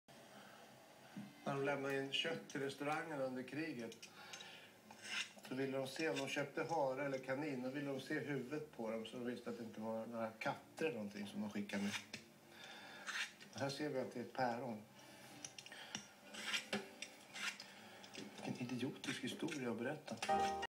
Per Morberg pratar om döda djur...
Per berättar om hur restauranger serverade kött förr..,